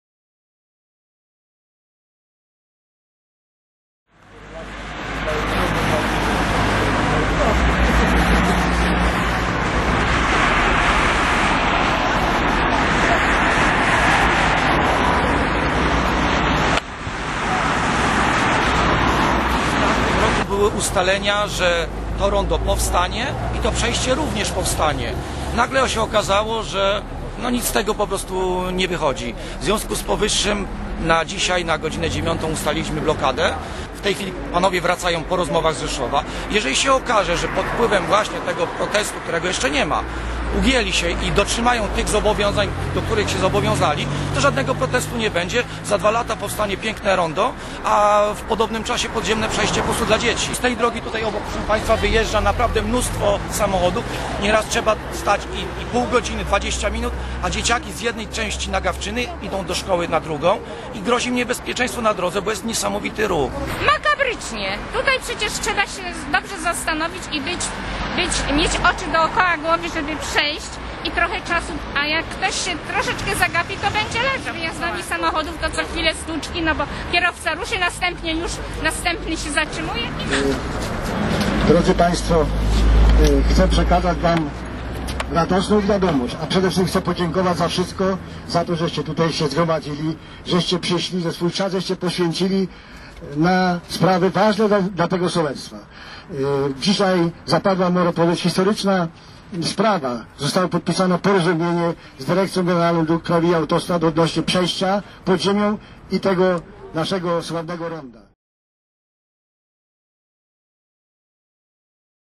Protest mieszkańców Nagawczyny
Do ostatniej chwili ważyły się losy dzisiejszego protestu na drodze krajowej nr 4. Opóźniającą się pikietę ostatecznie odwołano, gdy z negocjacji w Rzeszowie przyjechali przedstawiciele gminy Dębica i komitetu protestacyjnego.